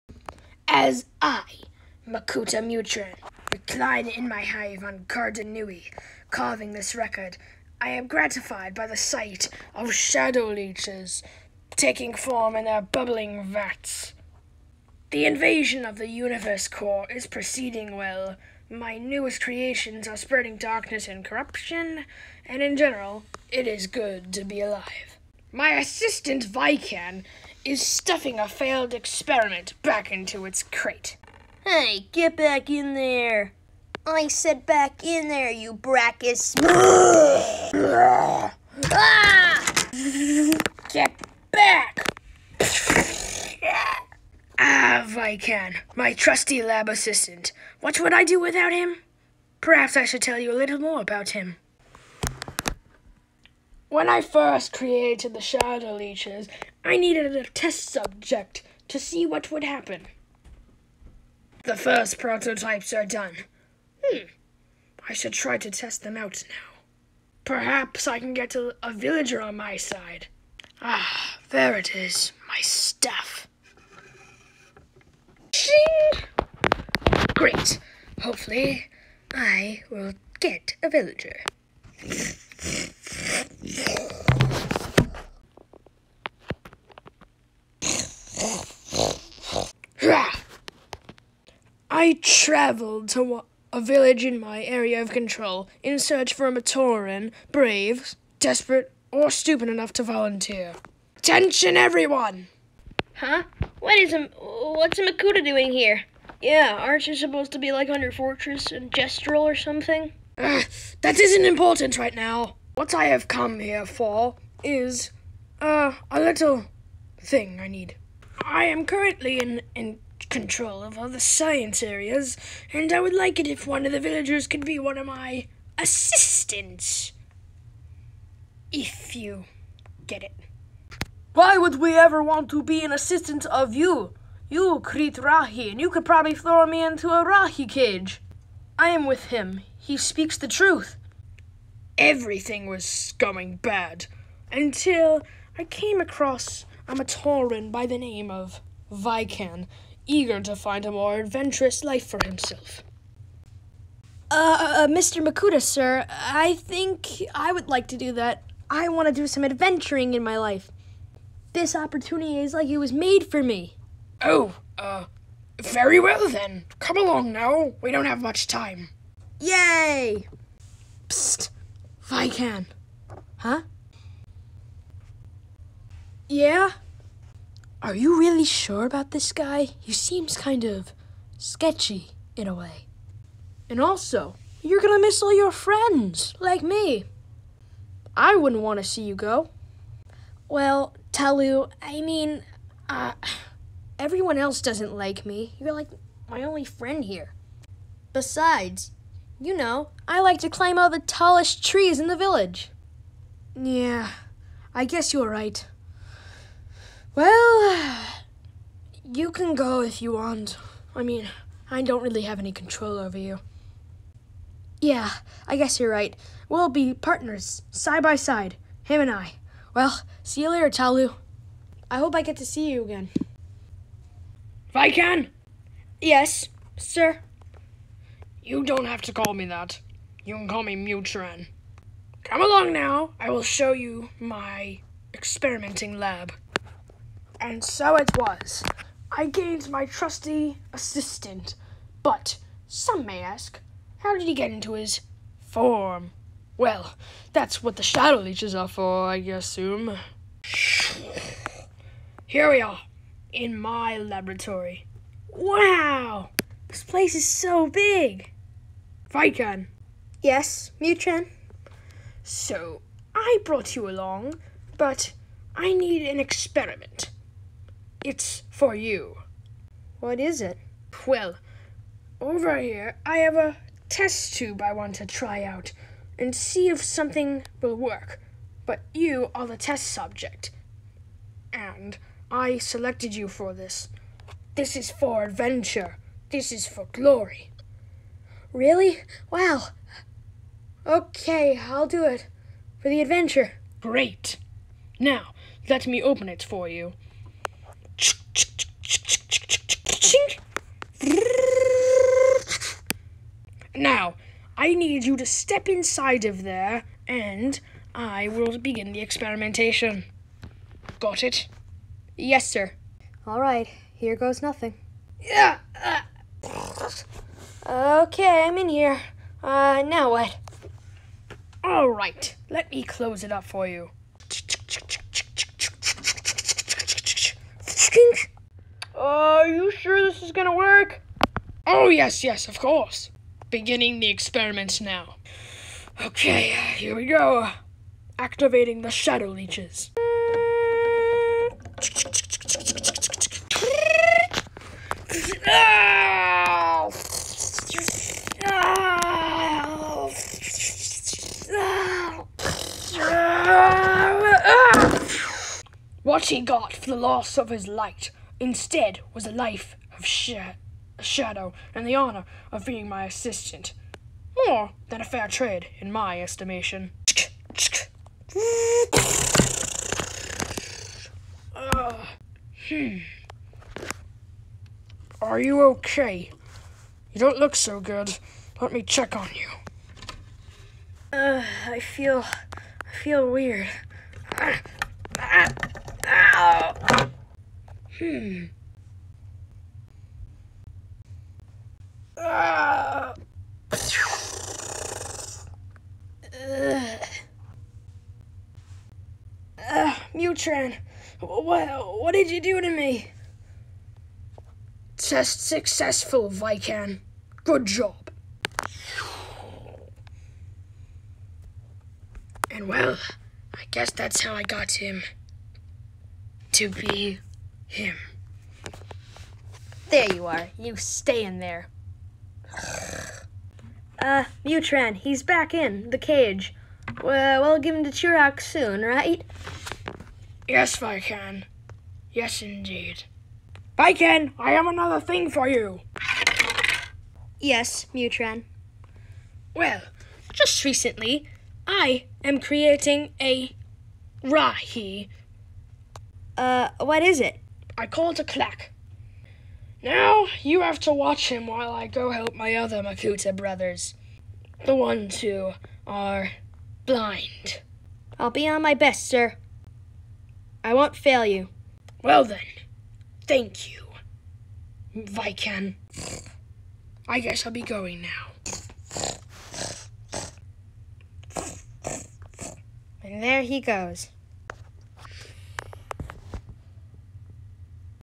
Bionicle- The Origin of Vican
This is the story of the unstable scientist Mutran and his lab Assistant Vican and how they came to be. I hope you guys enjoy!